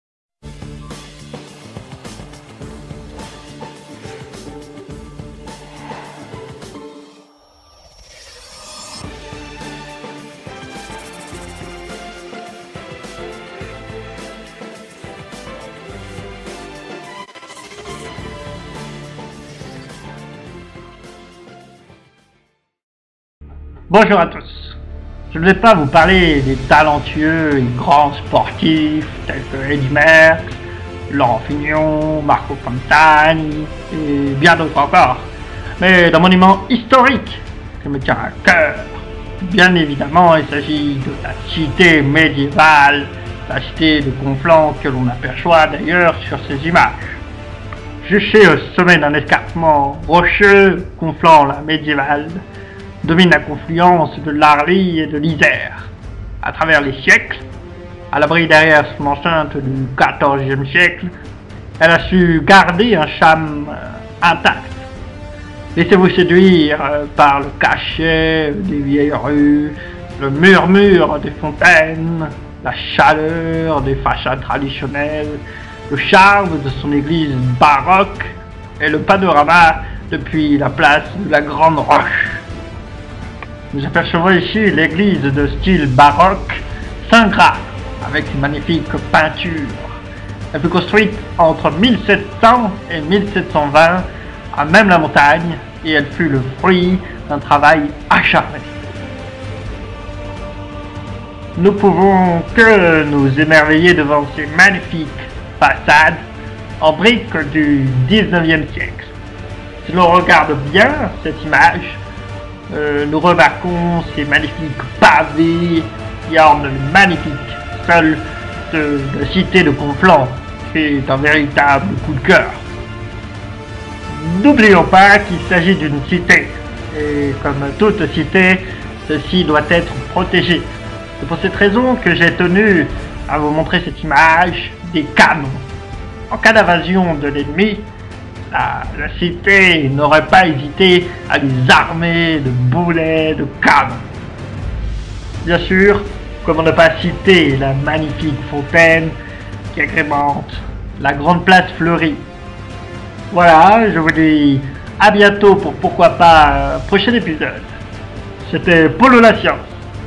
Parodie Jean Paul Ollivier - Cité Médiévale De Conflans
Imitation et parodie de M. Jean Paul Ollivier (commentateur TV sur le tour de France). J'espère que vous prendrez autant de plaisir à regarder cette vidéo que j'ai eu à la faire! Et vive Paulo la science!